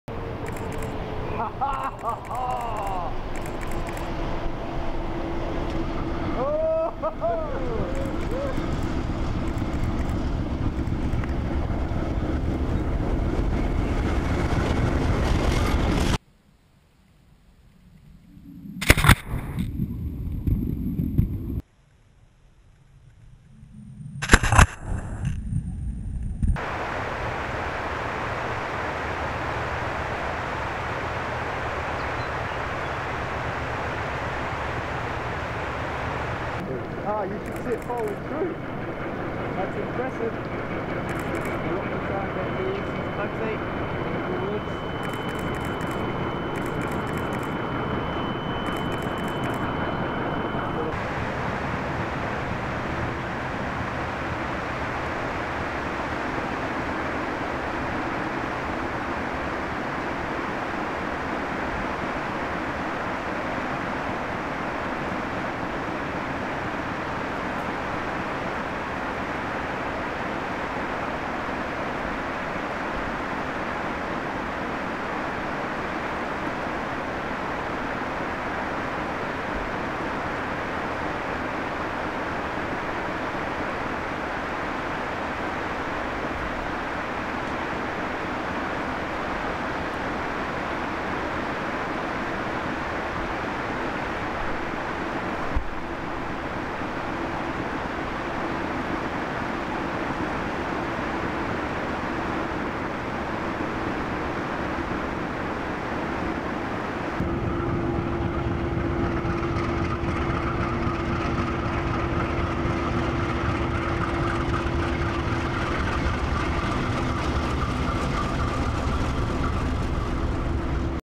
Spectacular footage Train plowing through deep snow Arthurs Pass
Kiwirail attempting to plow a route through to Christchurch today from Arthurs Pass made for some spectacular images.  07 June 2012